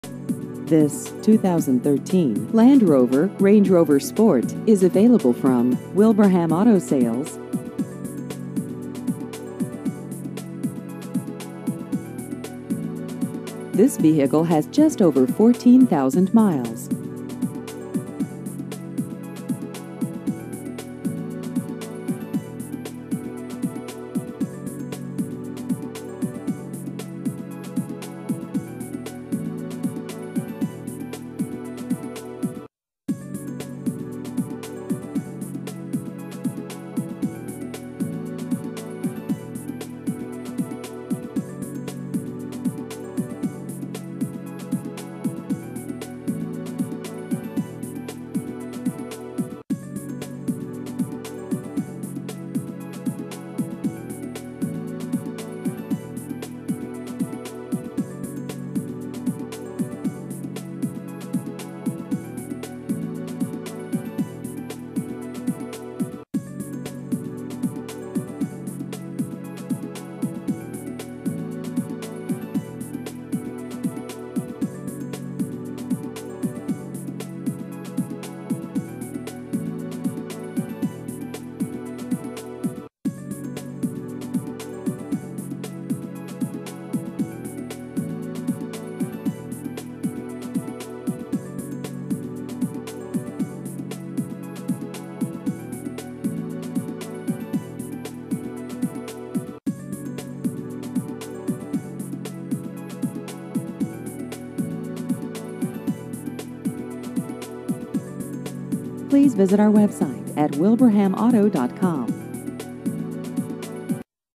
2013 Land Rover Range Rover Sport Used Cars Wilbraham ma
This 2013 Land Rover Range Rover Sport is available from Wilbraham Auto Sales.